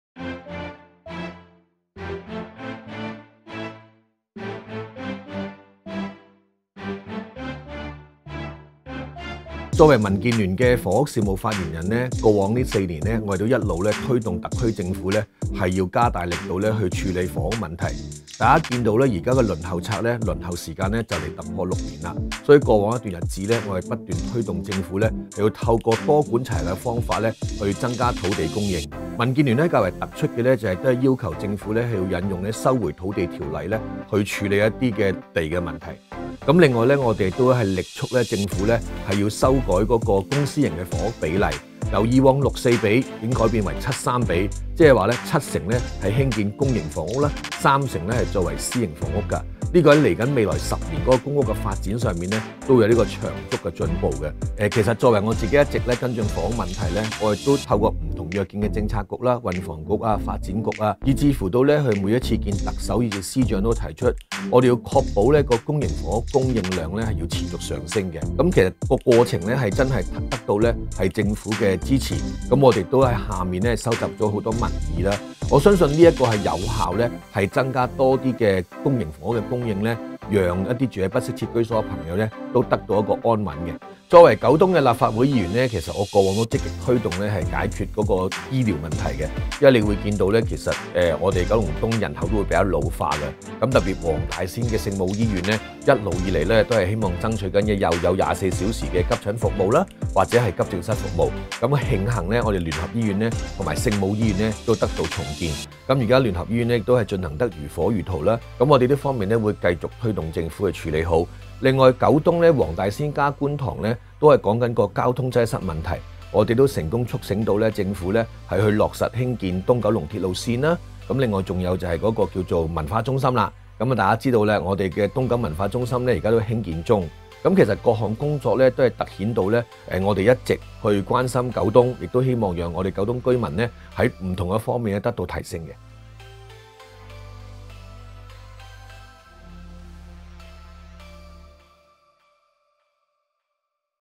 【建聞專訪】民建聯柯創盛：政府要多管齊下覓地建屋(3/6/2020)
【柯仔倡政府　多管齊下覓地建屋】

人稱「柯仔」的立法會議員柯創盛，一直關注市民居住問題。柯仔除促成政府將調整公私營房屋比例，令未來10年公營房屋供應大增外，民建聯亦一直要求政府善用《土地收回條例》這把「尚方寶劍」，多管齊下地增加土地供應。

請訂閱@OrChongShing